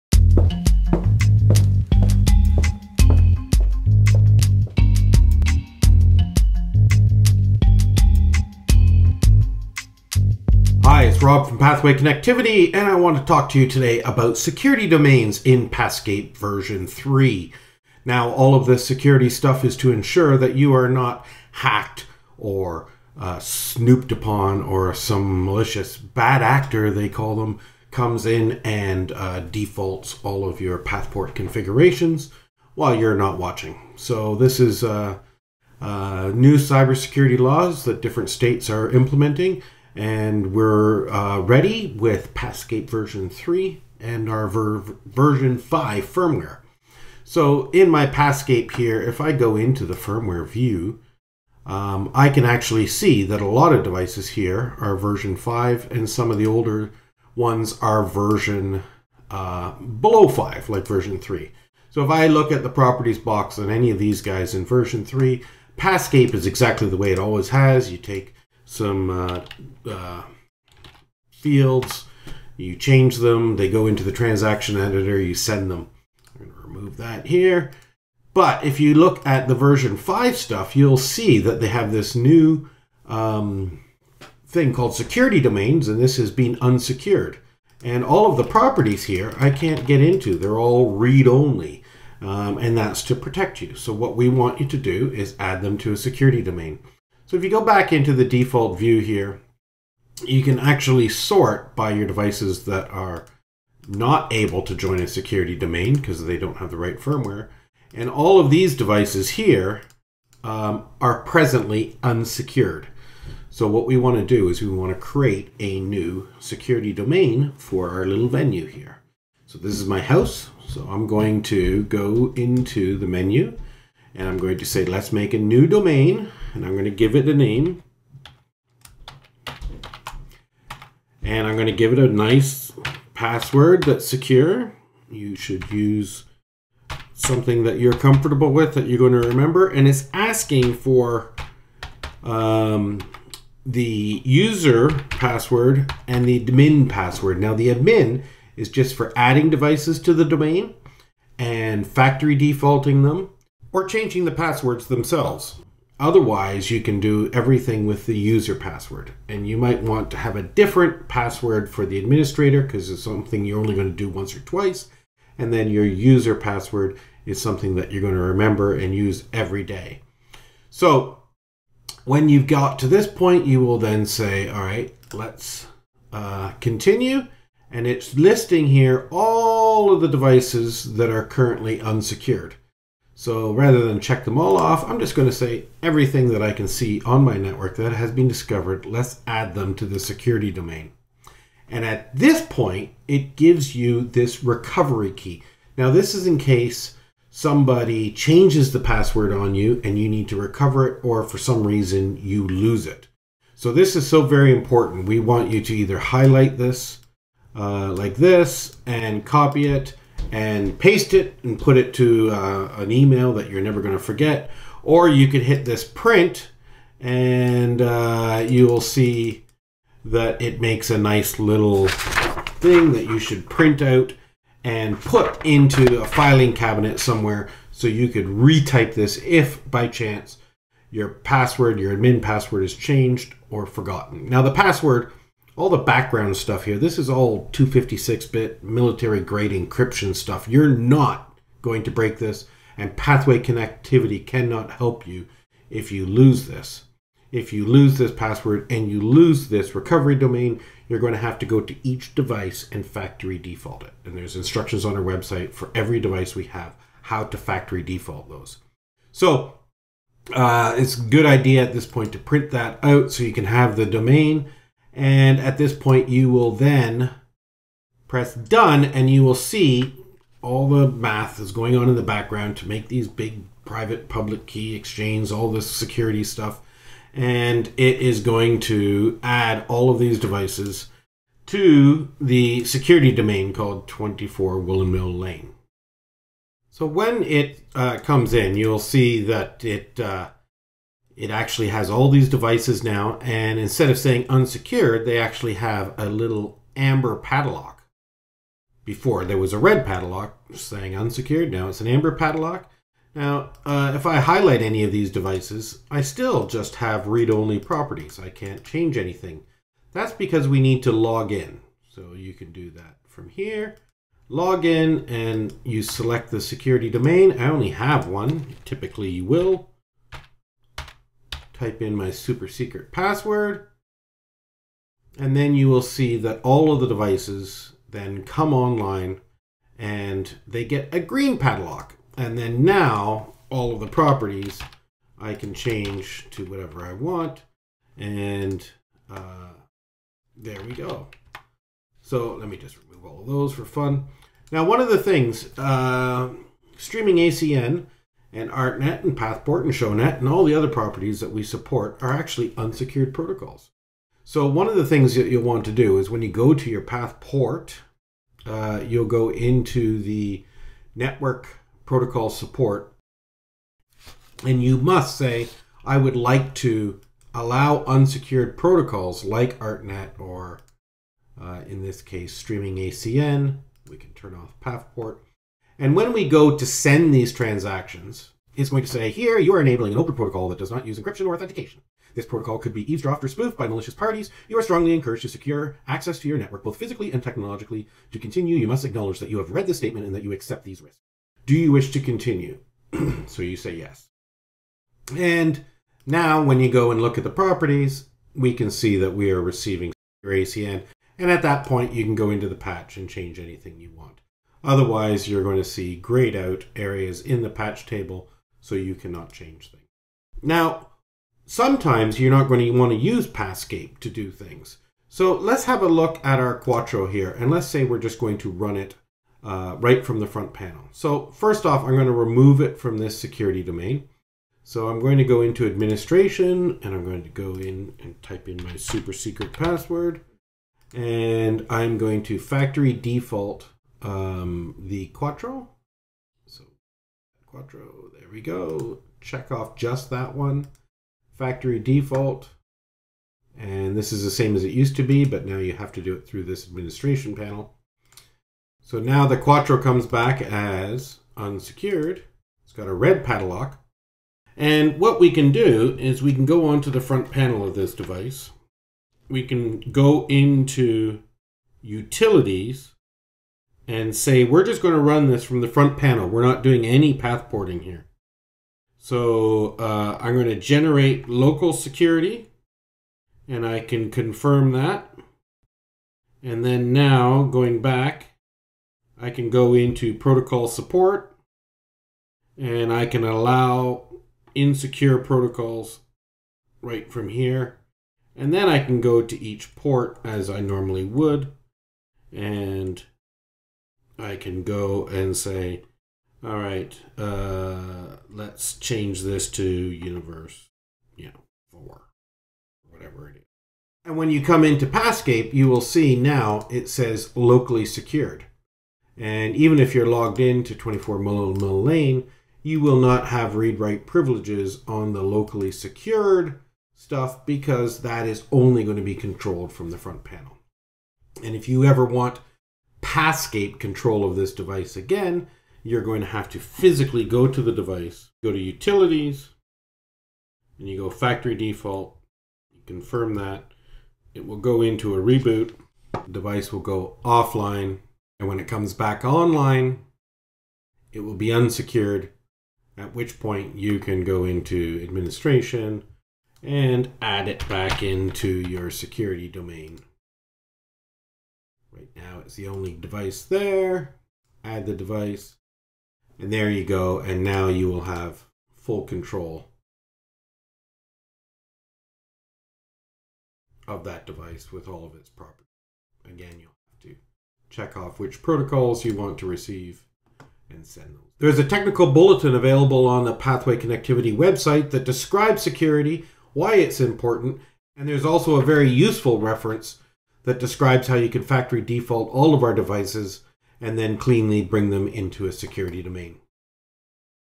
Pathscape 3: Security Domains
Overview on creating Security Domains, adding devices, factory defaulting and Local Security for use by front panel only.